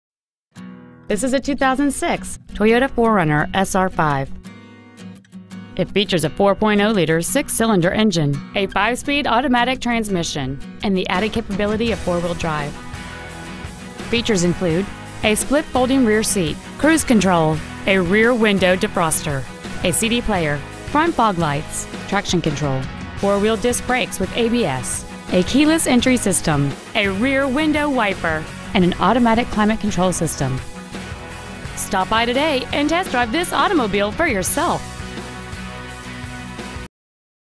2006 Toyota 4Runner Denver CO 80221
We've been honored to serve the Denver CO area , we promise that your experience at our dealership will exceed your expectations ! Year : 2006 Make : Toyota Model : 4Runner Engine : 4.0L V-6 cyl Trans . : 5-Speed Automatic Exterior : Gray Miles : 116,373 Interior : Gray Mountain States Toyota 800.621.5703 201 W. 70th Ave. Denver , CO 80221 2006 Toyota 4Runner Denver CO